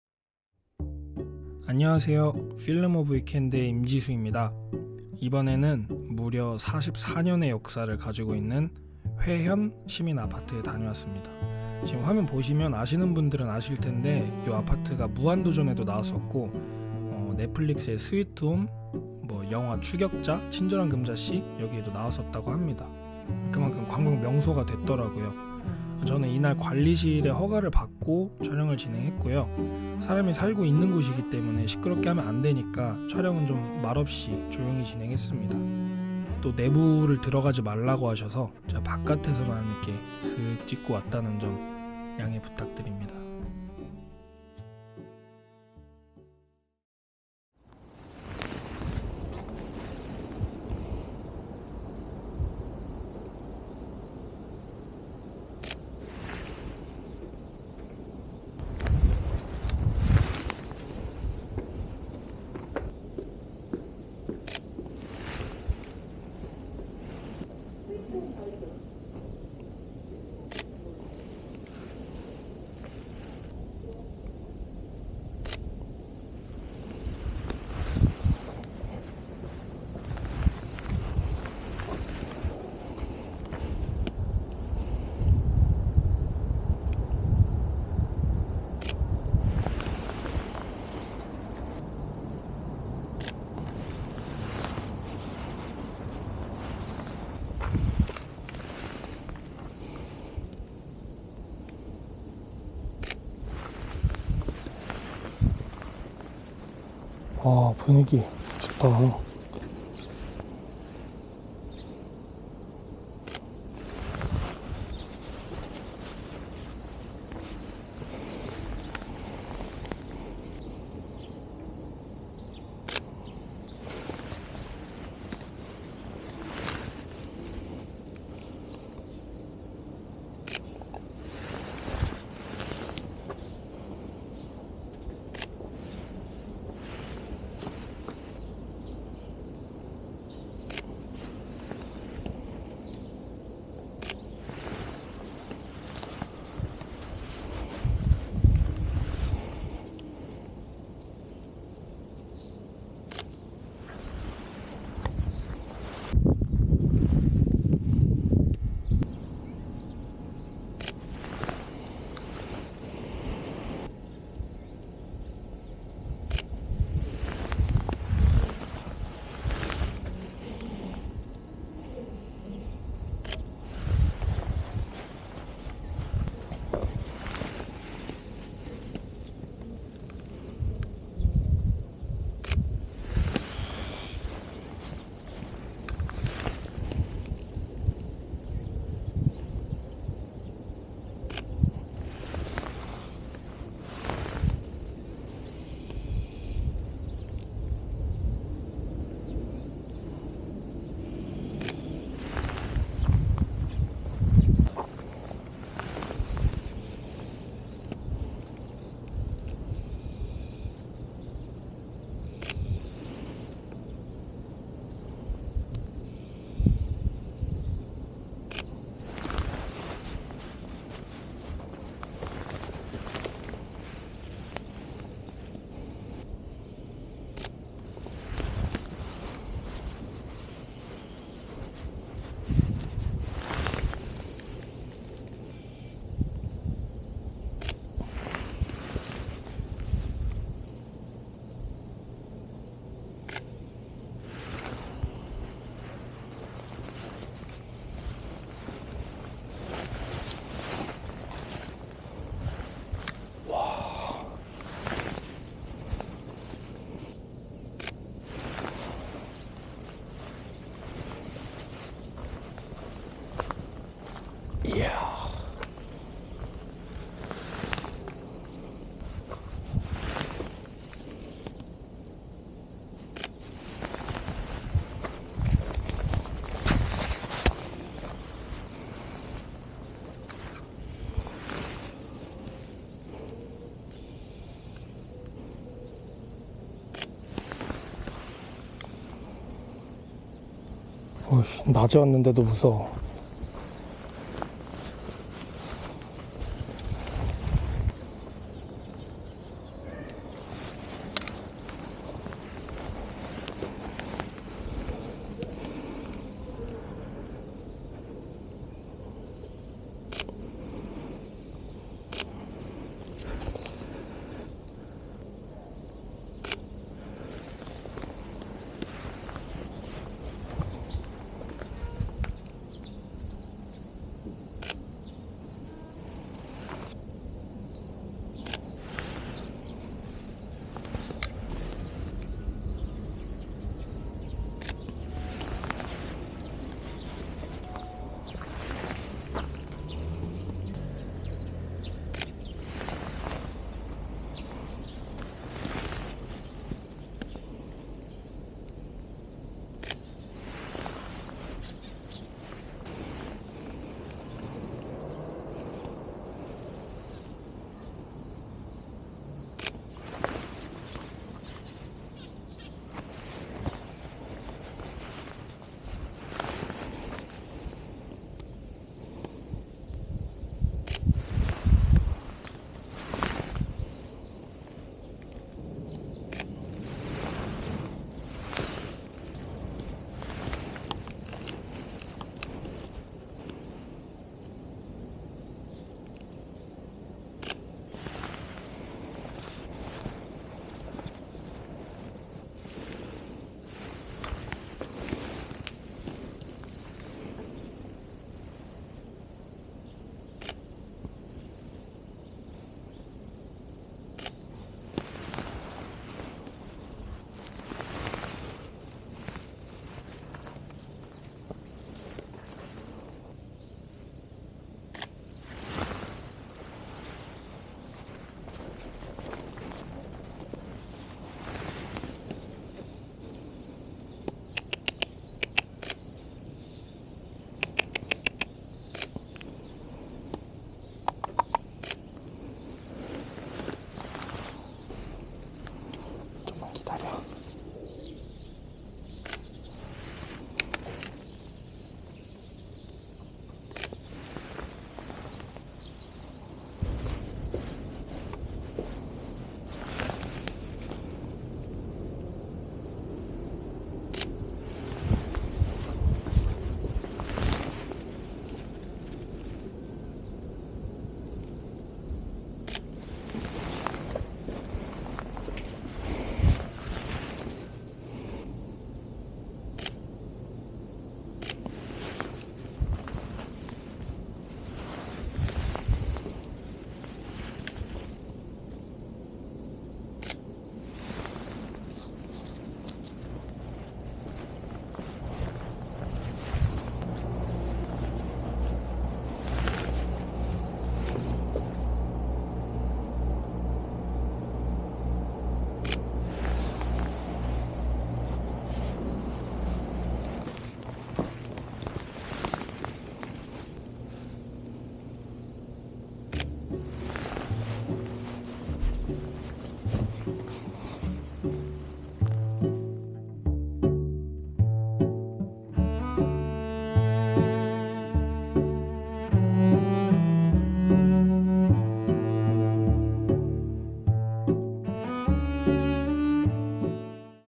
진짜 오래된 아파트를 촬영하고 왔습니다 | 40년이 넘는 역사를 가진 회현제2시민아파트 | 남산타워 | street photography
안녕하세요!
Film of weekend의 임지수입니다✌️

👉Main camera
👉POV Camera

[Music]
Artlist 유료 음원

영상 속 사진들 및 저의 일상을 볼 수 있습니다😃

오늘의 영상 속 장소는
남산 인근에 있는 회현제2시민아파트입니다!
[서울 중구 퇴계로8길 101]